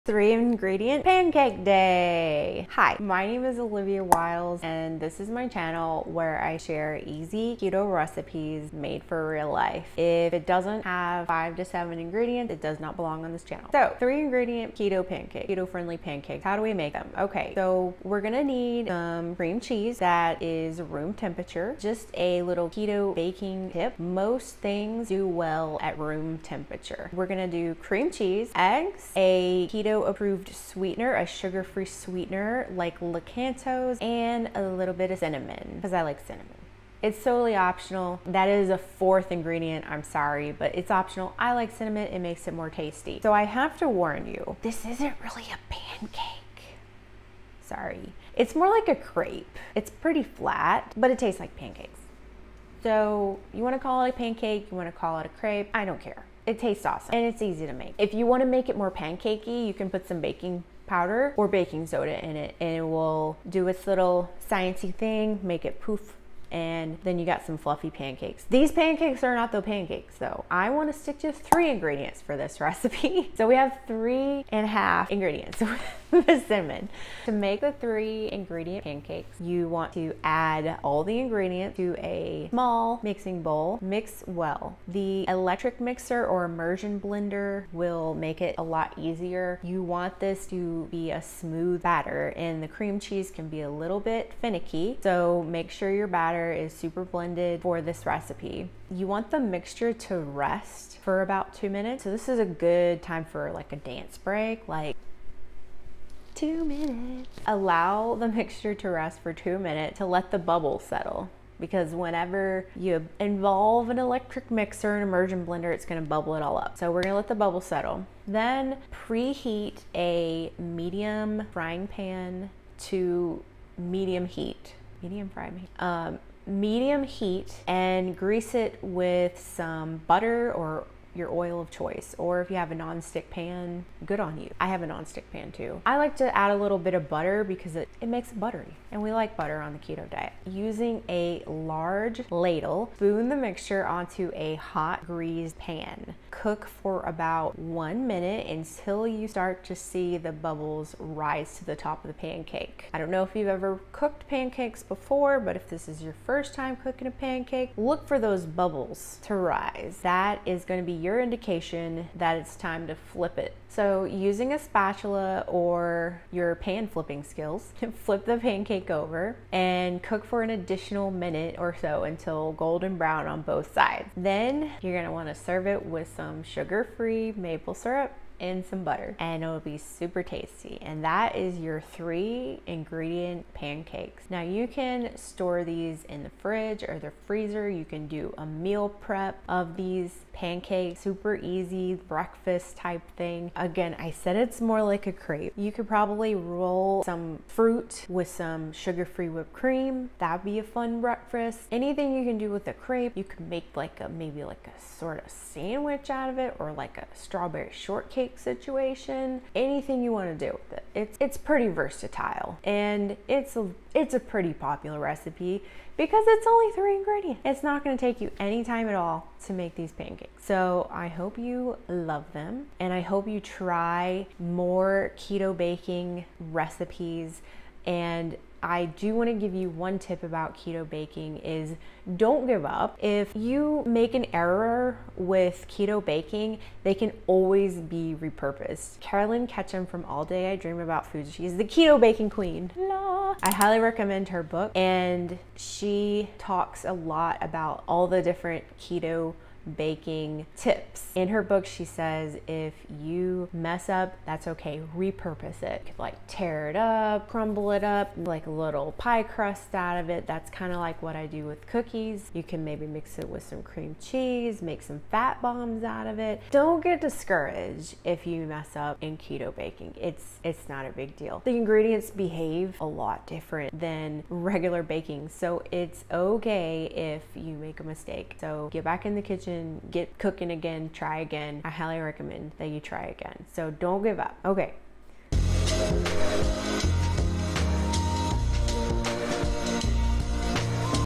Keto 3 Ingredient Pancakes | Breakfast | Carnivore | High Protein | Low Carb | Sugar Free
Keto 3 Ingredient Pancakes | Breakfast | Carnivore | High Protein | Low Carb | Sugar Free | Gluten Free | Easy Pancakes

Ingredients
2 ounces cream cheese
2 eggs
1 packet or tsp sugar-free sweetener
1/2 teaspoon cinnamon (optional, but so tasty)

Instructions
Add all ingredients to a small mixing bowl. Mix well. Electric mixers or immersion blenders make it easier.
Allow mixture to rest 2 minutes to let the bubbles settle.
Preheat a medium frying pan to medium heat and grease with butter or oil of choice.
Using a large ladle, spoon mixture onto a hot greased pan. Cook about 1 minute until you start seeing bubbles rise to the top of the pancake.
Using a spatula or your pan flipping skills, flip the pancake over. Cook for an additional minute or so until golden brown on both sides.
Serve with sugar free maple syrup and butter.

Makes about 2-4 pancakes. 4 net carbs for recipe.

Timestamps:

0:00 - Introduction
0:02 - About my channel
0:14 - About this recipe
0:43 - A word of caution!
0:57 - How to make it fluffy
1:17 - How to make 3 ingredient keto pancakes
2:54 - How to serve
3:04 - How to store
3:13 - Stuff you can pair with 3 ingredient pancakes
3:48 - Keto baking & repurpose tips

Carolyn Ketchum's book: The Ultimate Guide to Keto Baking

Amazon storefront